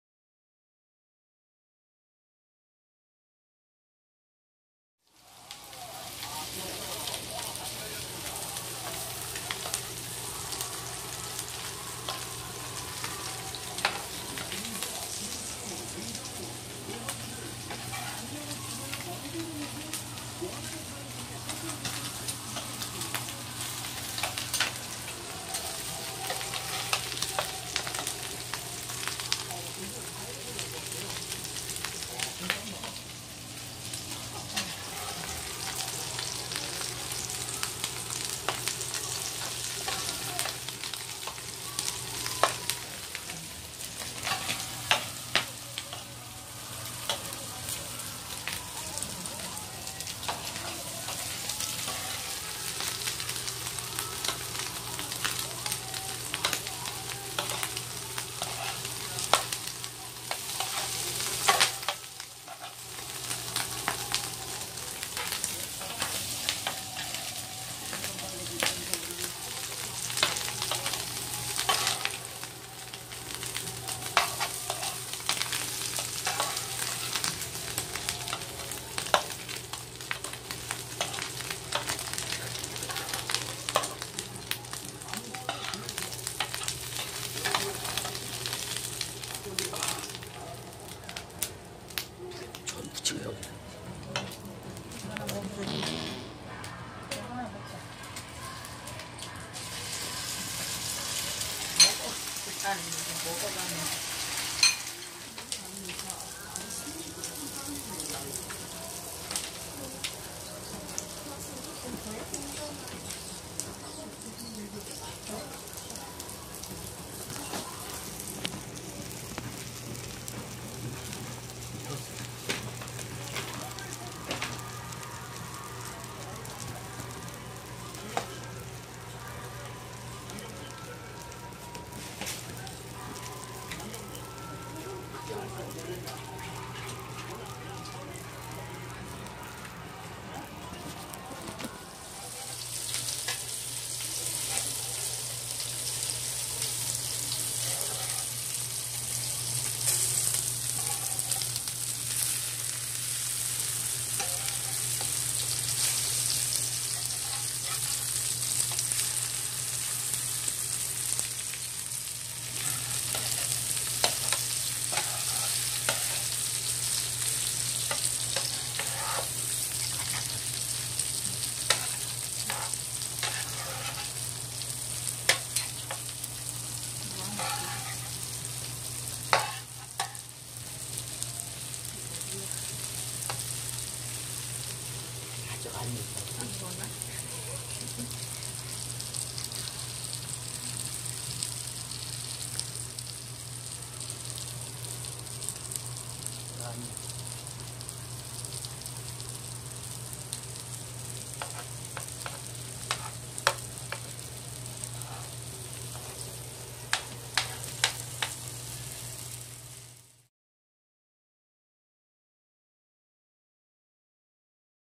명진tv 설.명절 음식을 만드는 모습입니다.( 꾸벅 )
안녕하세요 명진tv 입니다 여러분의♡구독 ♡은제게 큰힘이 됩니다  오늘은 설.명절 음식 만든는 모습 공개합니다  많은 관심 부탁드립니다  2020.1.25